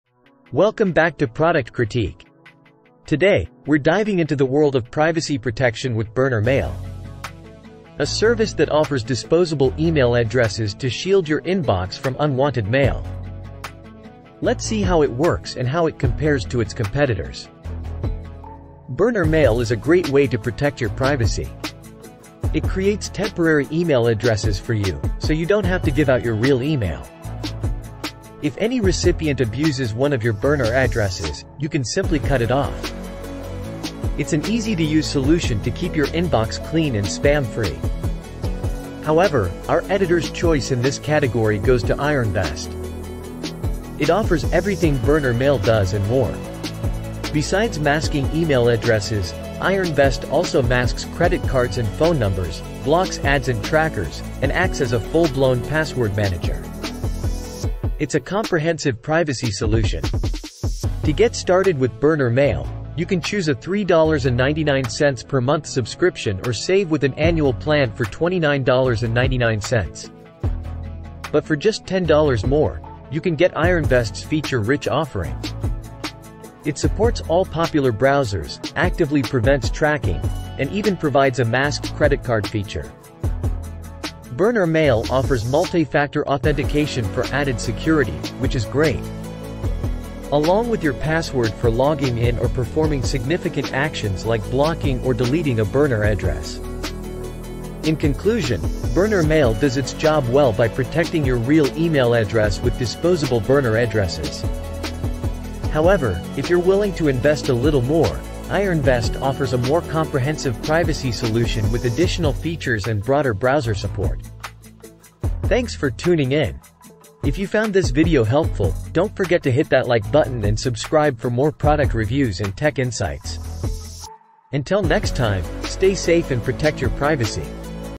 Burner Mail Review: Protect Your Privacy!
Welcome to our Burner Mail Review! In this video, we delve into the world of online privacy and security by exploring the features and functionality of Burner Mail. Join us as we demonstrate how this powerful tool lets you create temporary and anonymous email addresses to protect your personal information from spam, phishing attempts, and data breaches. We'll walk you through its easy setup process, customizable options, and seamless integration with various platforms. Watch the review and discover how to take control of your online identity.